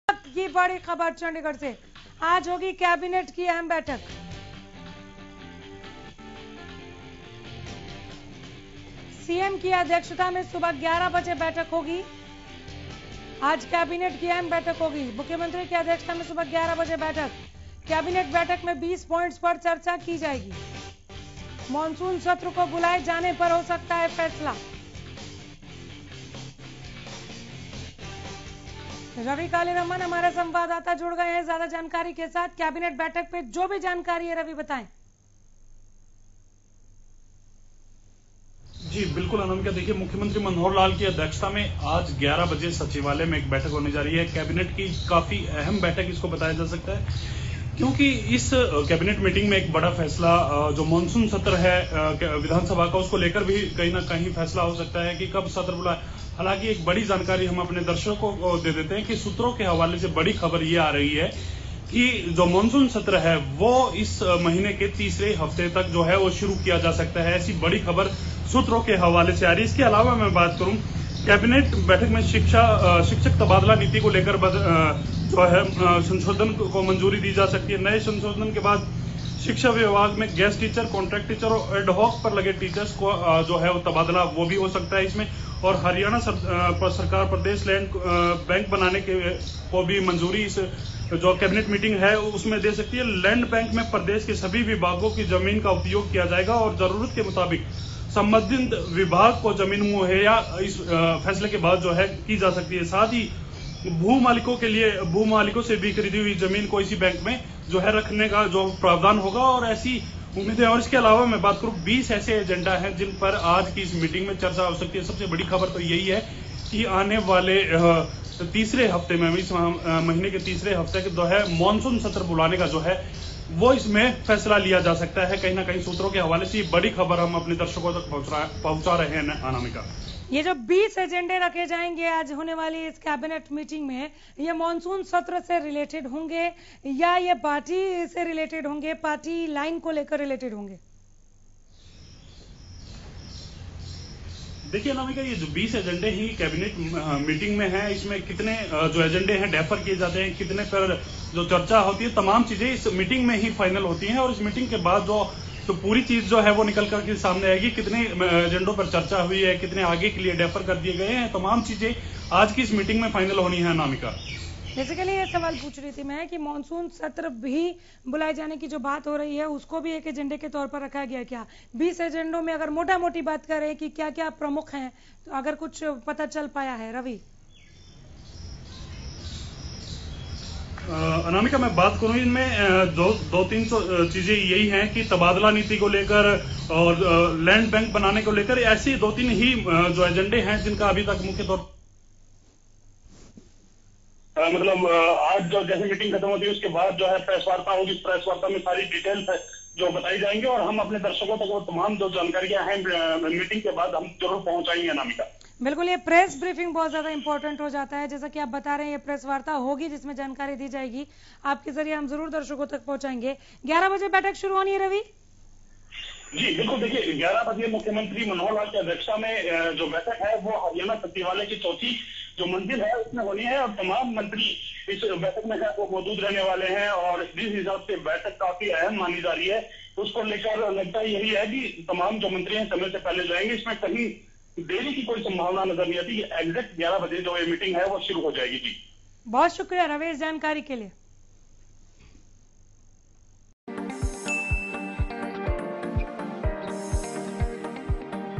कैबिनेट की अहम बैठक जारी, जानिए क्या कुछ होगी खास ? || STV Haryana News
कैबिनेट की अहम बैठक जारी, जानिए क्या कुछ होगी खास ?